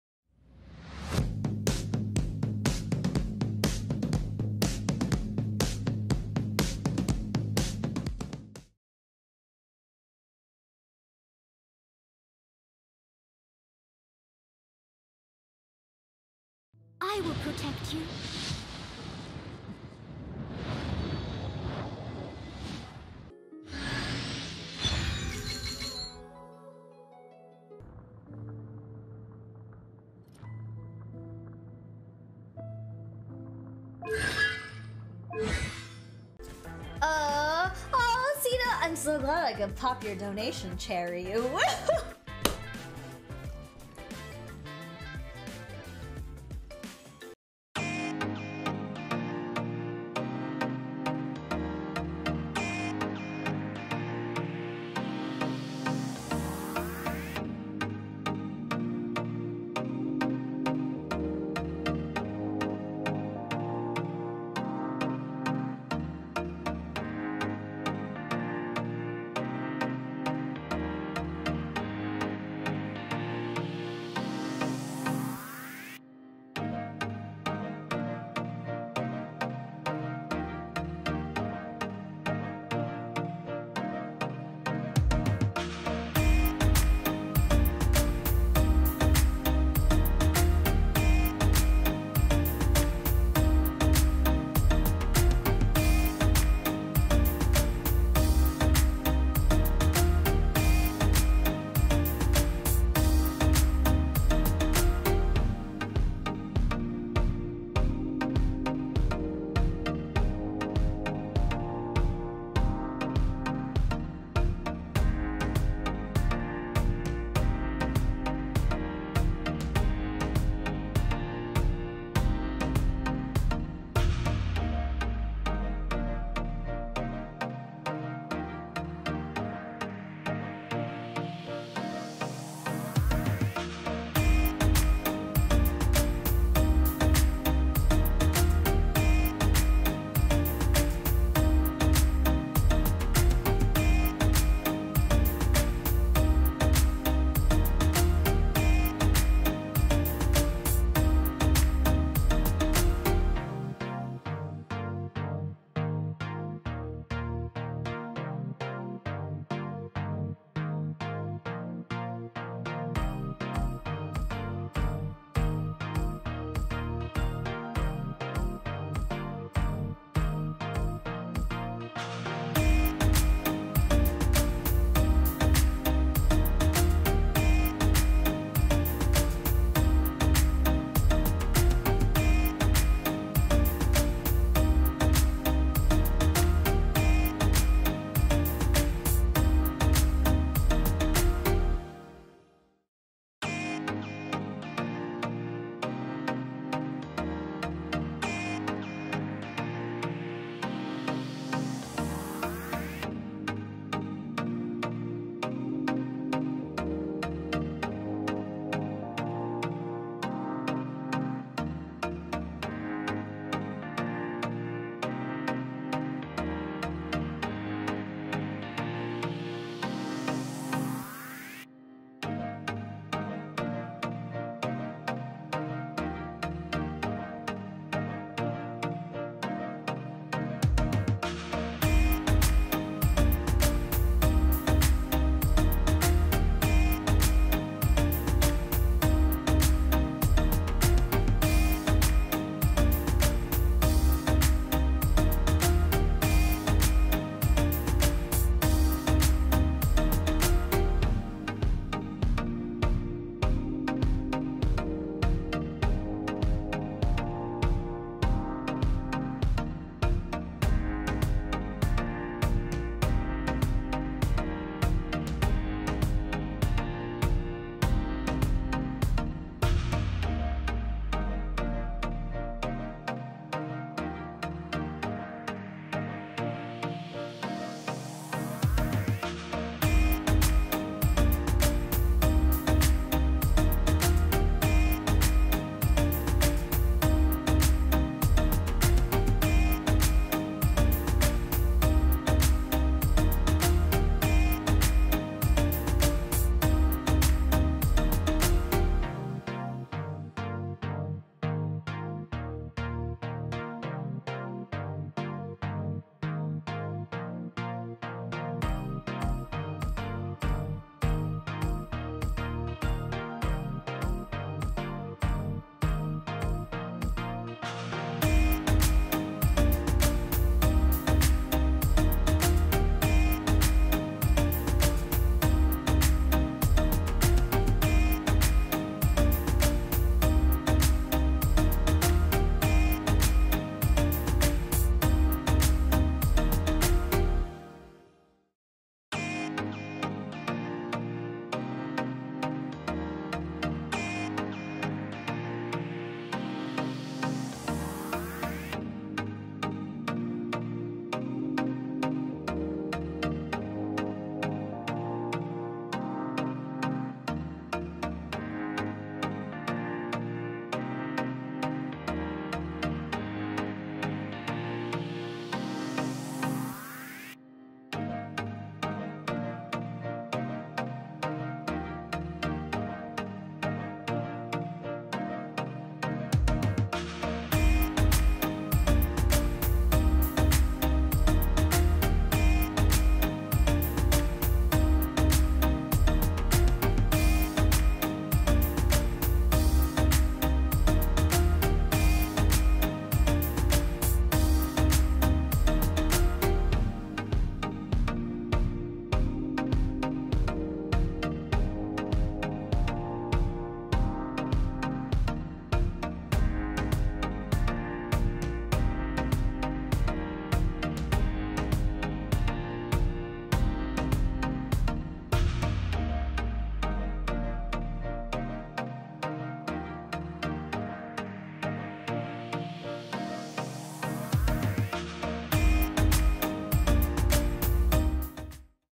Noelle Cosplay Beginner EASY BODY PAINT Tutorial 💡 Variety Content Ideas # 4
EASY BEGINNER BODY PAINT TUTORIAL of Noelle from Genshin Impact! 💖 When you can't find ready-to-wear cosplay or cosplaying armored characters, try body paint! 🎨 Makes for awesome content too!

Sorry for the delay with videos, cough has been lingering for weeks now! Really need to see a doctor but I'll be back publishing weekly videos again soon 💖

Special shout out to SeeKaySee! 🥰

Contents of this video:
0:00 - Intro
0:17 - Inspiration
0:46 - What You Need
1:14 - Preparations
1:39 - Base Paint
3:26 - Add Shadows
4:02 - Add Outlines
4:58 - Contour
5:53 - Finishing Touches